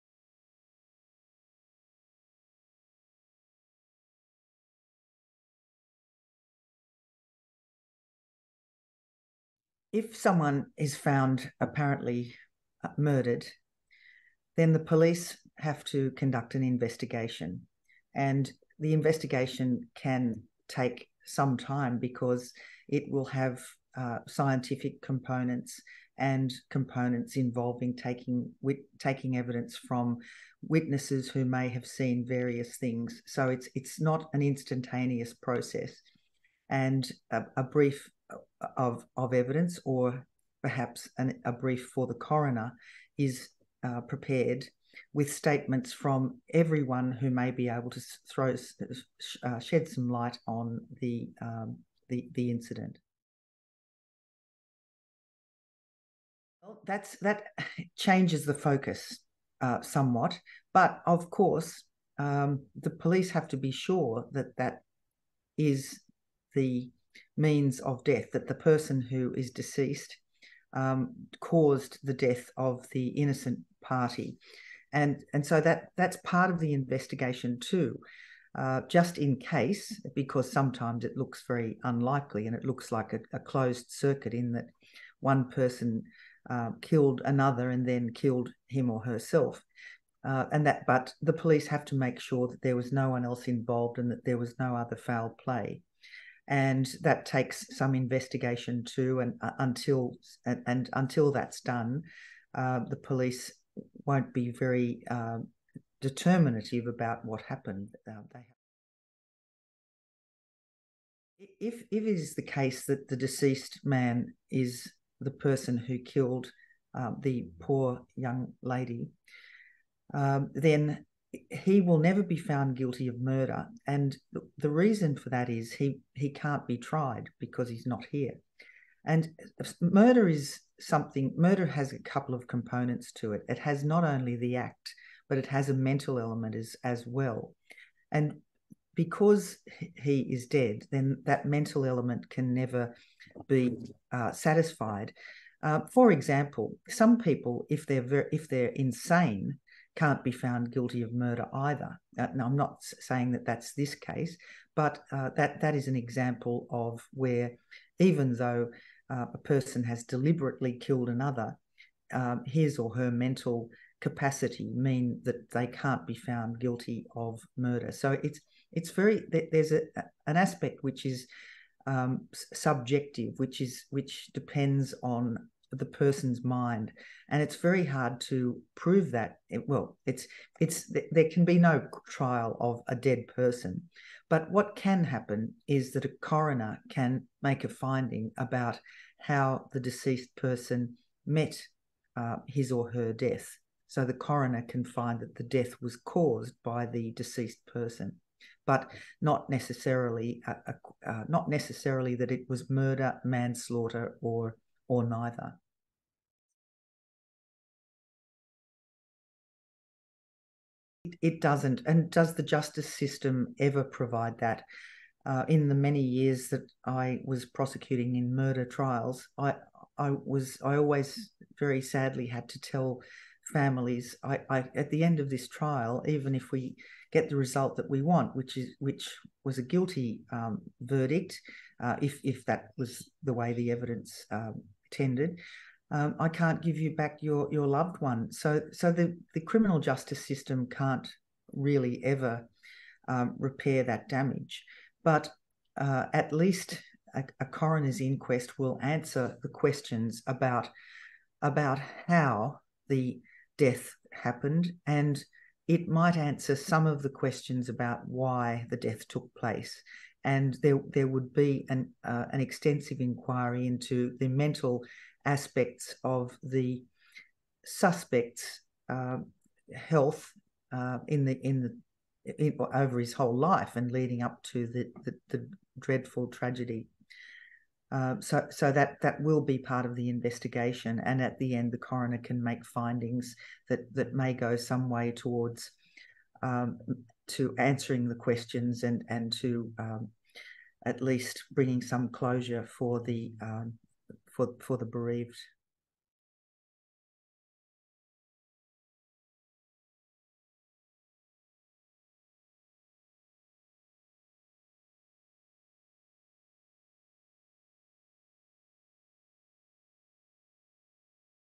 Criminal Investigation Process Explained: Margaret Cunneen SC
Leading Criminal Law Senior Counsel Margaret Cunneen SC discusses the Criminal Investigation process in a murder where the suspect dies before for the trial, whether a deceased person can be found guilty of murder, the role of the coroner's court and whether justice is provided to victims and families